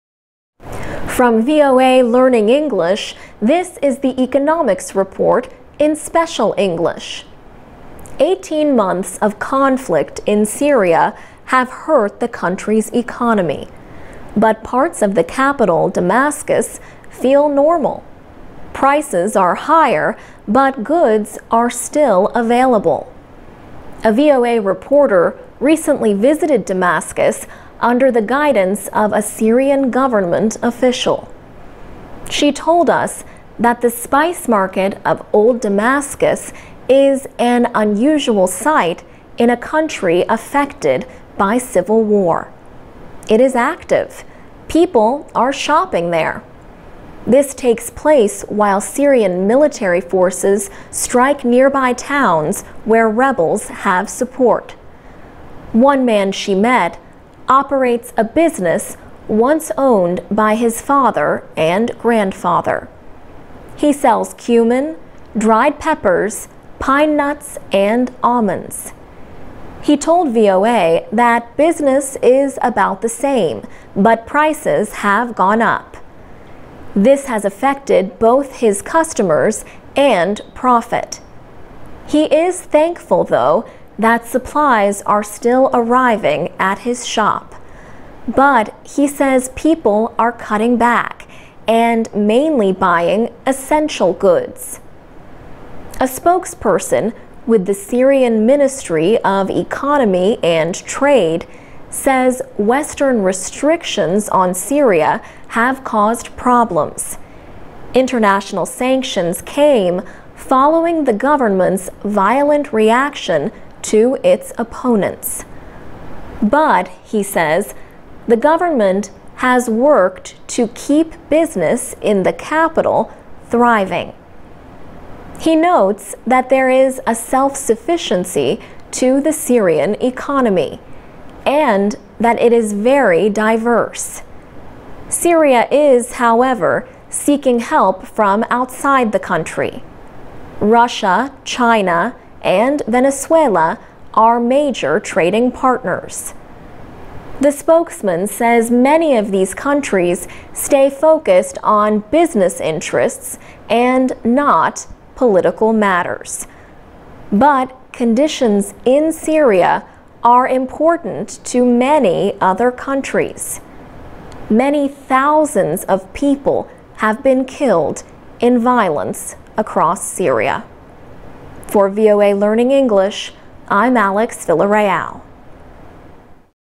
War in Syria Stays Out of Damascus Spice Market
From VOA Learning English, this is the Economics Report in Special English.Eighteen months of conflict in Syria have hurt the country's economy.  But parts of the capital, Damascus, feel normal.  Prices are higher.  But goods are still available. A VOA reporter recently visited Damascus under the guidance of a Syrian government official.  She told us that the Spice Market of Old Damascus is an unusual sight in a country affected by civil war.  It is active. People are shopping there.  This takes place while Syrian military forces strike nearby towns where rebels have support. One man she met operates a business once owned by his father and grandfather. He sells cumin, dried peppers, pine nuts and almonds.  He told VOA that business is about the same, but prices have gone up. This has affected both his customers and profit. He is thankful, though, that supplies are still arriving at his shop. But he says people are cutting back, and mainly buying essential goods. A spokesperson with the Syrian Ministry of Economy and Trade says Western restrictions on Syria have caused problems.  International sanctions came following the government's violent reaction to its opponents.  But, he says, the government has worked to keep business in the capital thriving.  He notes that there is a self-sufficiency to the Syrian economy.  And that it is very diverse.Syria is, however, seeking help from outside the country. Russia, China and Venezuela are major trading partners. The spokesman says many of these countries stay focused on business interests and not political matters.But conditions in Syria are important to many other countries. Many thousands of people have been killed in violence across Syria.For VOA Learning English, I'm Alex Villarreal.